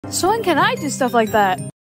Infinite Powers Clip: Rai's first appearance!
Apologies for it being a short clip, I want to show that I am working on the episode, which comes out in 63 days.

Voice actor is AlexisCC!
Program is Flipaclip, backgrounds are done in Ibis Paint X.